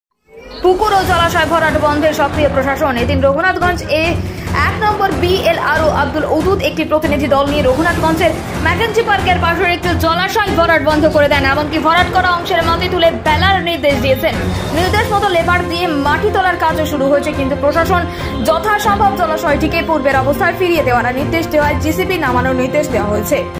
পুকুর ও জলাশয় ভরাট বন্ধের সক্রিয় প্রশাসন জঙ্গিপুরে।
Video from MD Masud ali